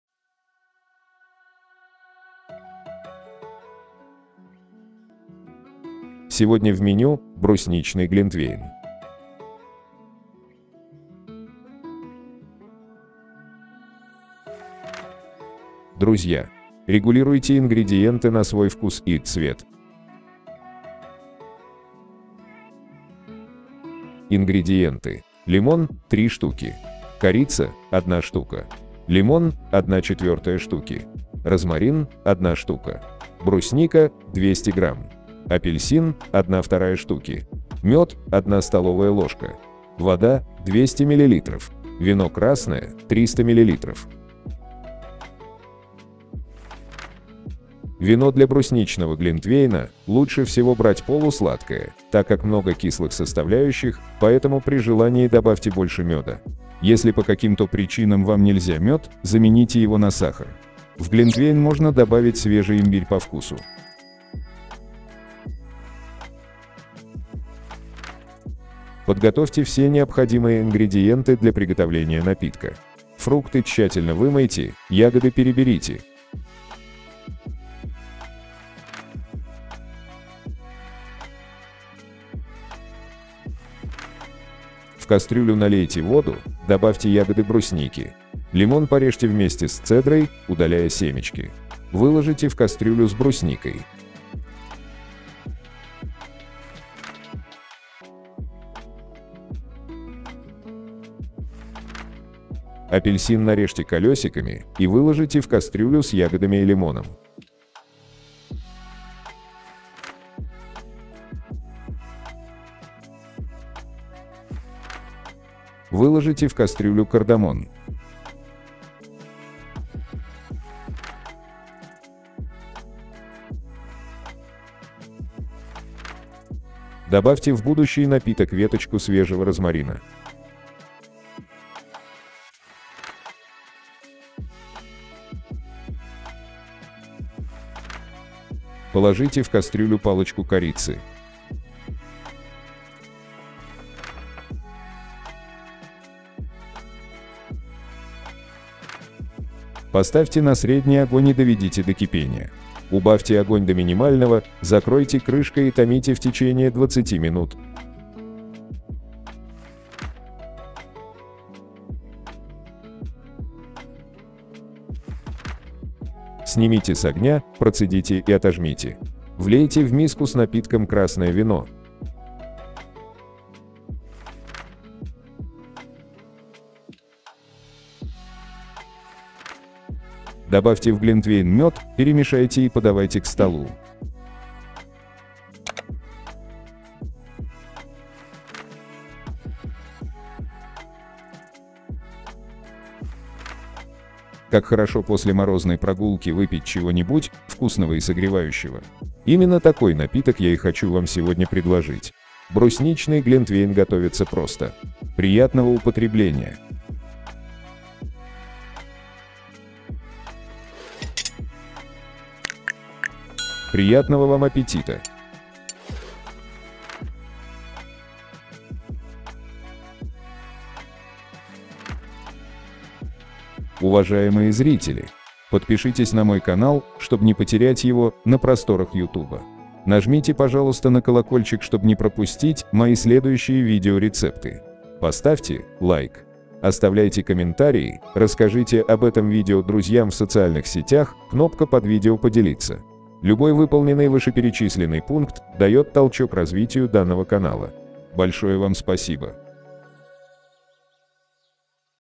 Брусничный Глинтвейн / Книга Рецептов / Bon Appetit
✅ Брусничный Глинтвейн 👇

Ингредиенты:
*лимон  — 3 Штуки
*Корица  — 1 Штука
*Лимон  — 1/4 Штуки
*Розмарин  — 1 Штука
*Брусника  — 200 Грамм
*Апельсин  — 1/2 Штуки
*Мед  — 1 Столовая ложка
*Вода  — 200 Миллилитров
*Вино красное  — 300 Миллилитров

Вино для брусничного глинтвейна лучше всего брать полусладкое, так как много кислых составляющих, поэтому при желании добавьте больше меда. Если по каким-то причинам вам нельзя мед, замените его на сахар. В глинтвейн можно добавить свежий имбирь по вкусу.

👉 Пошаговое приготовление 👇

1. Подготовьте все необходимые ингредиенты для приготовления напитка. Фрукты тщательно вымойте, ягоды переберите.

2. В кастрюлю налейте воду, добавьте ягоды брусники. Лимон порежьте вместе с цедрой, удаляя семечки. Выложите в кастрюлю с брусникой.

3. Апельсин нарежьте колесиками и выложите в кастрюлю с ягодами и лимоном.

4. Выложите в кастрюлю кардамон.

5. Добавьте в будущий напиток веточку свежего розмарина.

6. Положите в кастрюлю палочку корицы.

7. Поставьте на средний огонь и доведите до кипения. Убавьте огонь до минимального, закройте крышкой и томите в течение 20 минут.

8. Снимите с огня, процедите и отожмите. Влейте в миску с напитком красное вино.

9. Добавьте в глинтвейн мед, перемешайте и подавайте к столу.

10. Как хорошо после морозной прогулки выпить чего-нибудь вкусного и согревающего. Именно такой напиток я и хочу вам сегодня предложить. Брусничный глинтвейн готовится просто. Приятного употребления!